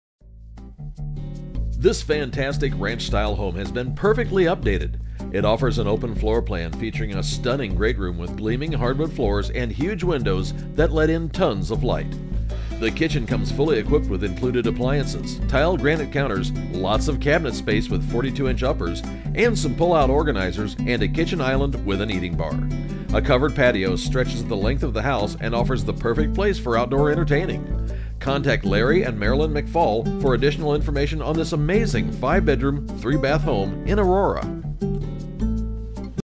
Home for Sale in Aurora, CO | $225,000
Address:  1761 Biscay Street
City:  Aurora
County:  Adams
State:  CO
Zip Code:  80011
Price:  $225,000
Bedrooms: 5
Total Baths: 3
Full Baths: 2
Partial Baths: 1
¾ Baths: 1
Garage Stalls: 4
Square Ft: 2705
Lot Size: 0.22
Year Built: 1978

This fantastic ranch style home has been perfectly updated.  With over 2,300 finished square feet, it offers an open floor plan featuring a stunning great room with gleaming hardwood floors and huge windows that let in tons of light.  The kitchen comes fully equipped with included appliances, tile granite counters, lots of cabinet space with 42&quot; uppers and some pull out organizers and a kitchen island with eating bar.  A covered patio stretches the length of the house and offers the perfect place for outdoor entertaining.  Large master bedroom with private 3/4 bath plus two more bedrooms and full bath on the main floor.  The finished basement has a large family room plus two more large bedrooms and a full bath.  Bonus! heated 16 x 24 workshop with electrical,. plus a utility shed.  Fenced yard, including gated RV parking space with room to park two RVs in tandem.  Upgrades  throughout!  Updated Electrical. Total cost of running auto fed pellet stove only $430 per year.

Larry McFall
Office: Southeast Metro at DTC